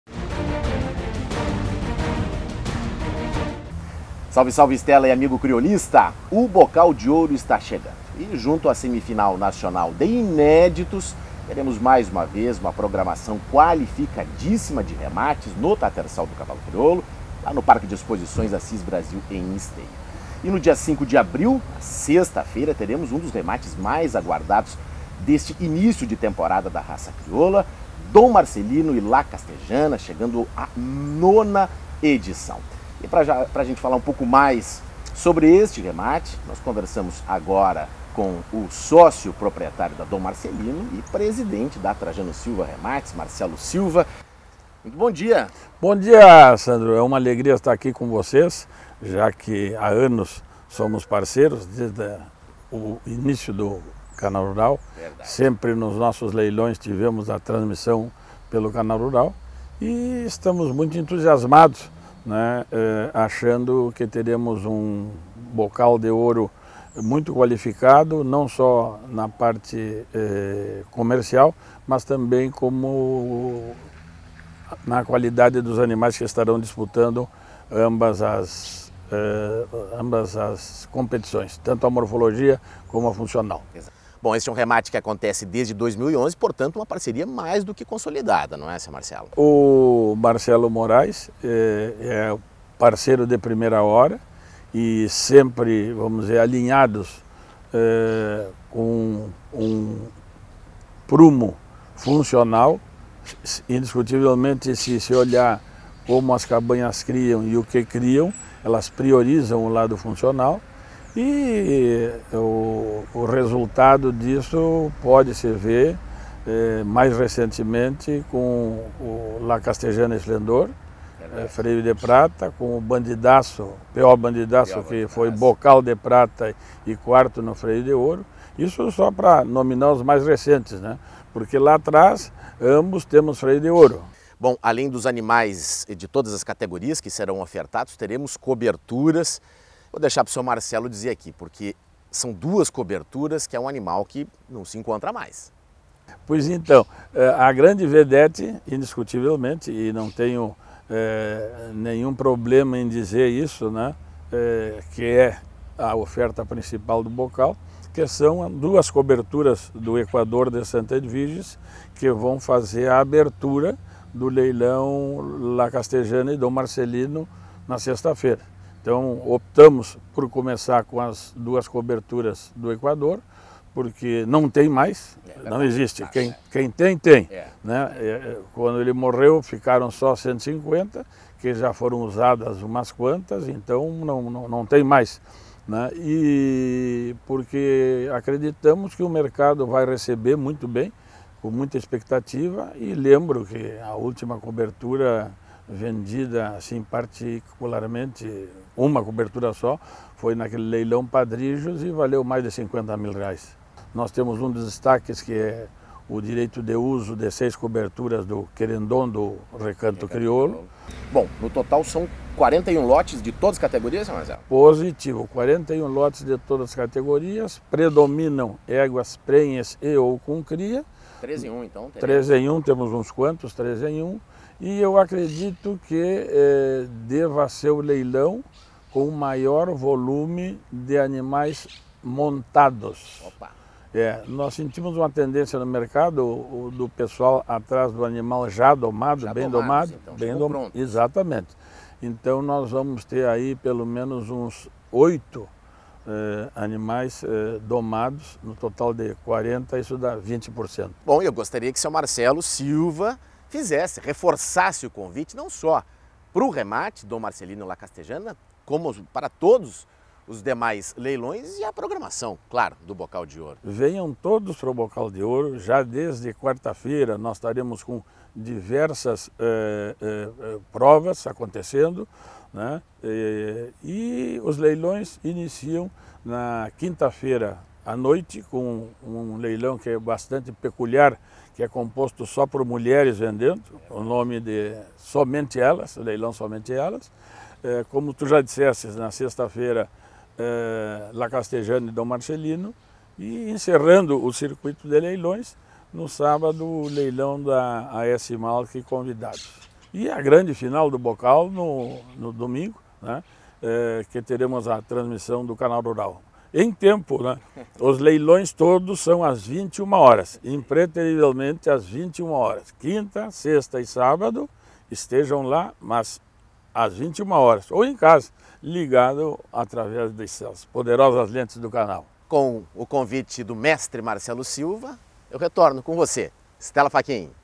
Vitrine: os destaques do remate da Don Marcelino & La Castellana
Veja no quadro Vitrine os destaques da oferta da Don Marcelino & La Castellana para o remate que será realizado no dia 5 de abril, dentro da programação do Bocal de Ouro 2019.